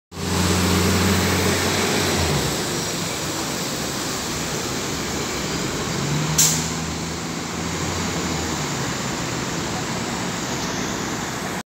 Línea 140 - E.R.S.A. Metalpar Iguazú III Mercedes Benz O 500 U
Vemos pasar por calle Corrientes al 900 de la ciudad autónoma de Buenos Aires al interno 3754 de la línea 140 E.R.S.A , un Metalpar Iguazú III O 500 U el martes 7 de septiembre de 2021 a las 11:14 hs.